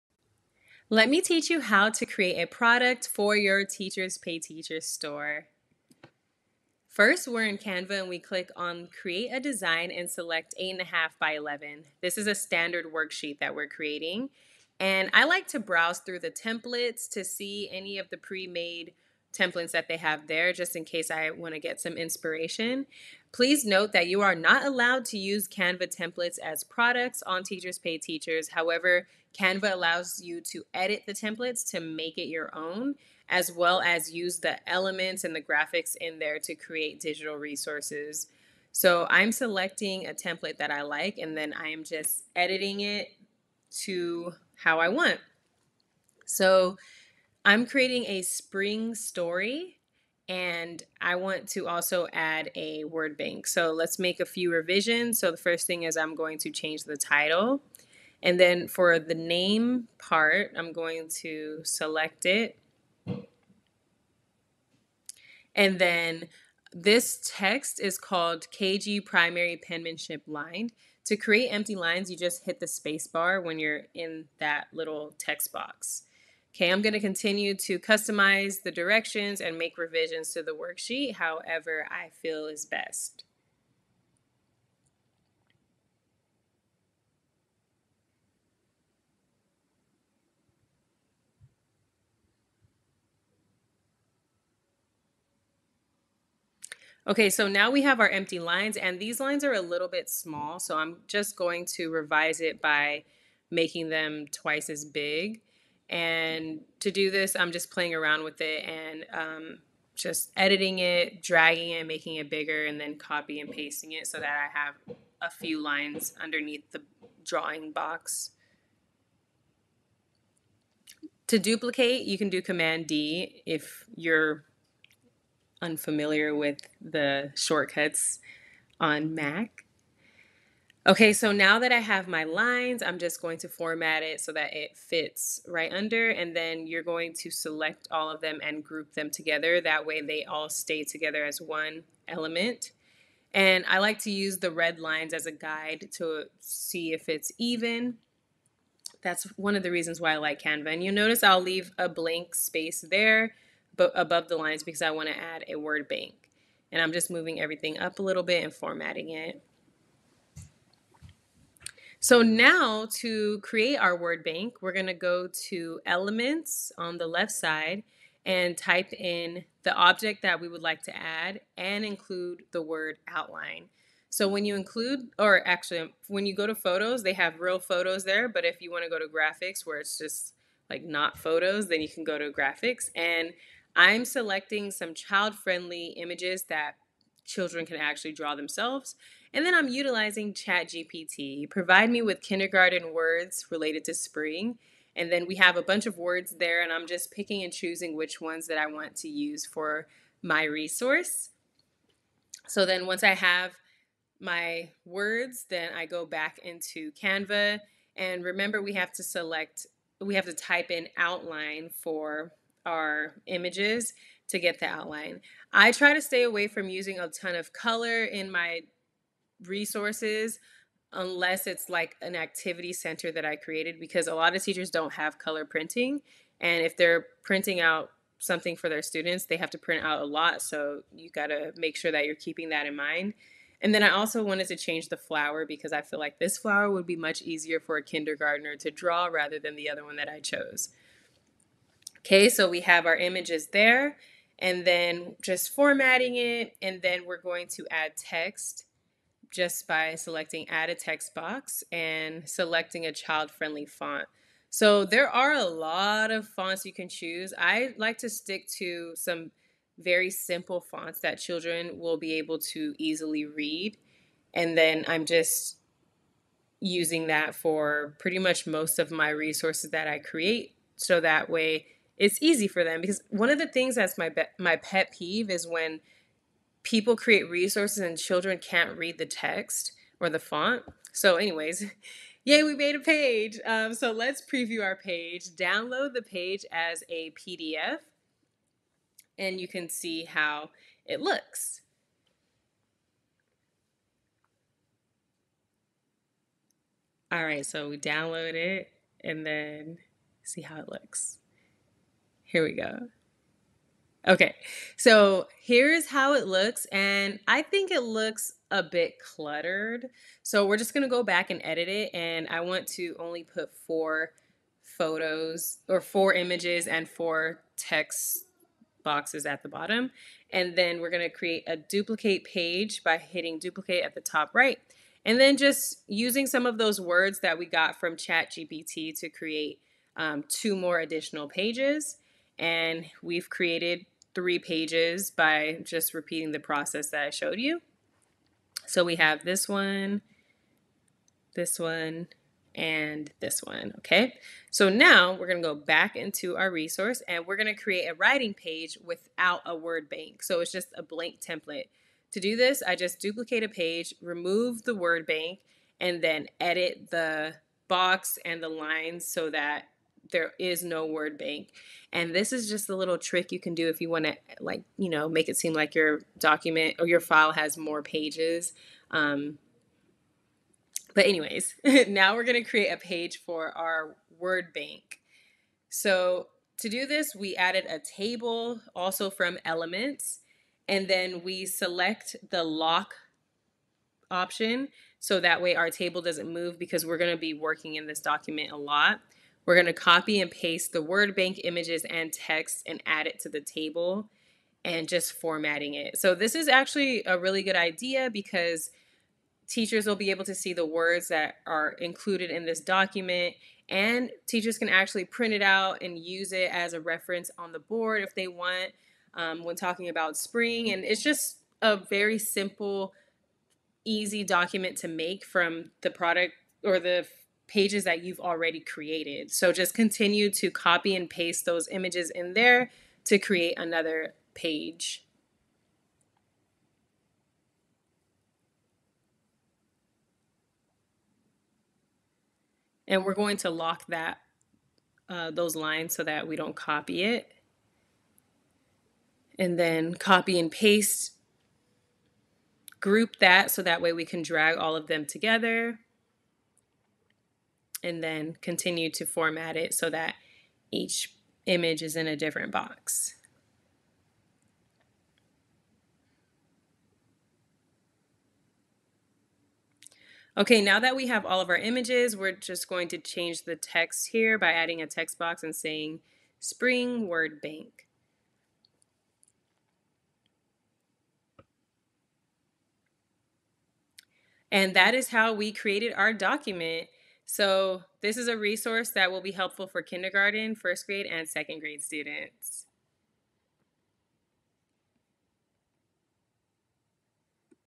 How to Make an Education Resource - Canva Tutorial for beginners
In this video I will be sharing how to create an education resource within Canva.